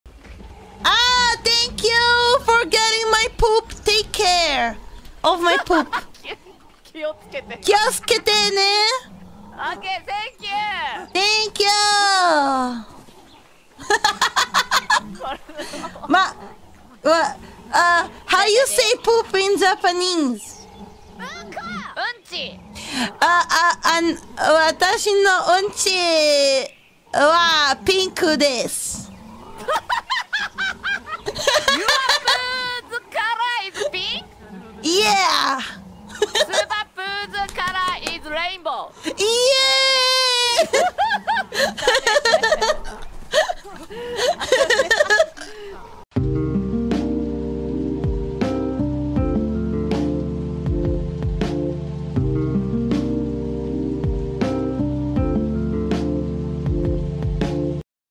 Raora getting her "unchi" helped by Subaru...【Hololive Clip】
Talents in the clip: @holoen_raorapanthera @OozoraSubaru
Thumbnail:

Hololive
Hololive ch
hololive reaction
hololive animation
hololive english
holoadvent
hololive advent
hololive council
hololive myth
Holocure

BGM used
DOVA-SYNDROME